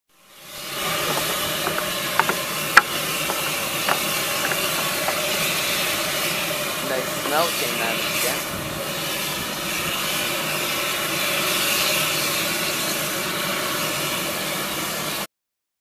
Red Spouter 2013
This is a video of the Red Spouter Fumarole within the Fountain Paint Pots area of Yellowstone National Park.  Video taken on 7/25/2013.